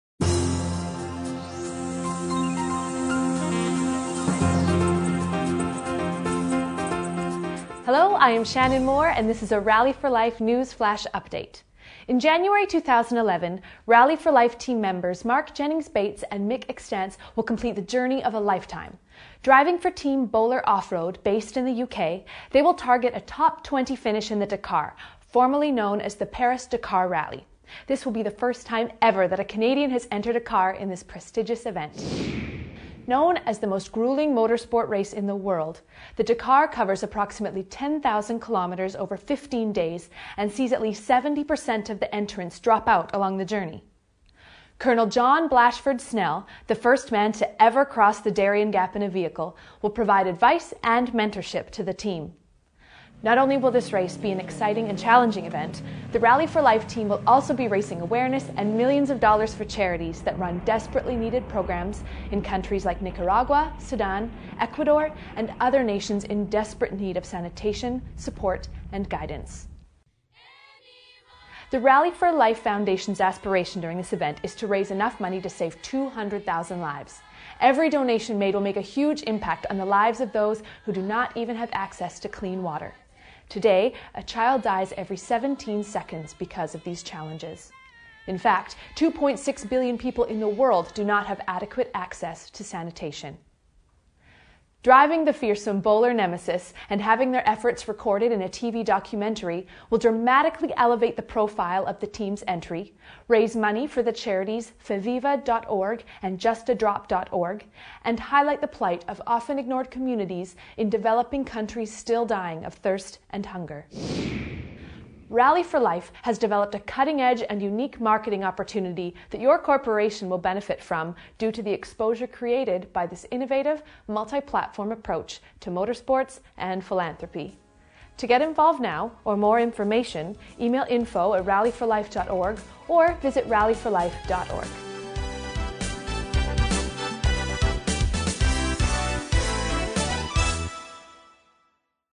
Rally4Life Dakar Commercial Promo movie
Join the Rally4Life team as they endeavour to be one of the top finishing teams in the 2011 Dakar Rally and raise $4 million to save 200,000 lives in the process. The project will be centered around a 6 part TV series outlining the worlds water shortage problems and the teams aspiration to compete in the Dakar.